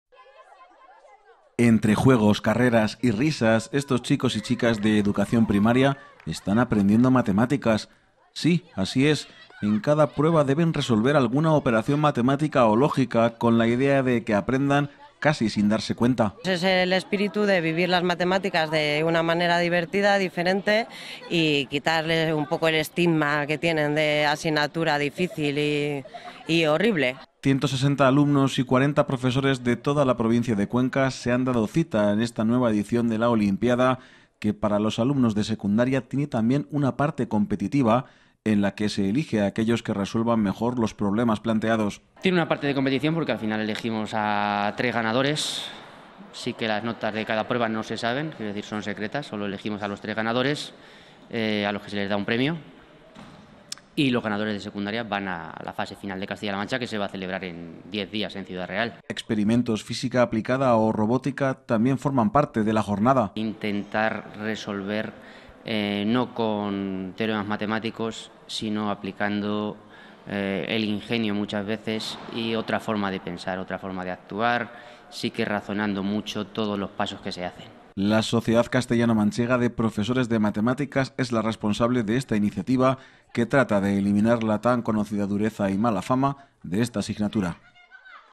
JUEGOS PARA ACERCARSE A LAS MATEMÁTICAS
Entre juegos, carreras y demostraciones prácticas se desarrolla la olimpiada matemática en la que participan alumnos de colegios e institutos de toda la Región. En Cuenca los chicos y chicas han podido disfrutar de una jornada lúdica, de un tipo de pruebas diferentes y de una manera original de completar su educación.